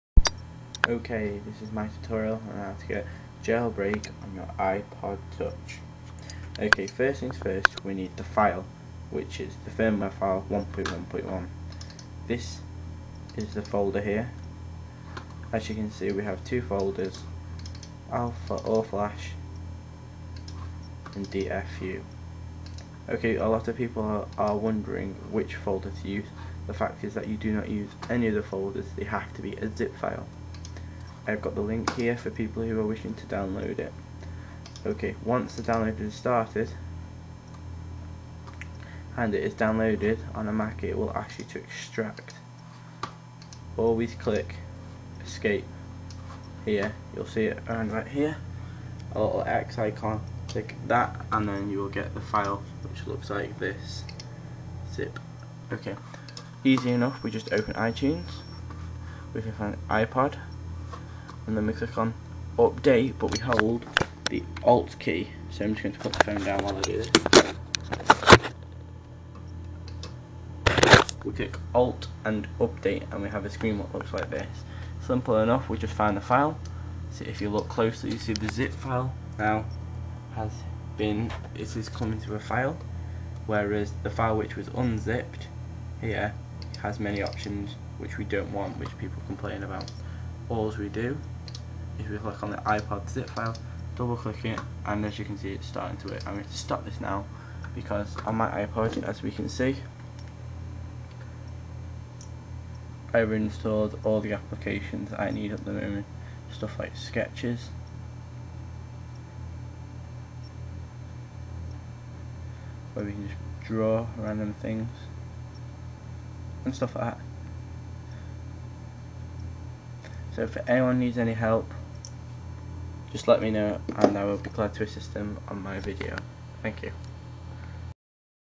How to get jailbreak on your ipod touch for Mac or Windows
This is my toutorial on how to get JailBreak on your ipod touch, using the 1.1.1 firmware.

I have done this video using the .zip file because allot of people are expanding the file when the
ipod cant read extracted files!

Any questions please ask, i will post new video answering questions!

The top file, its in blue!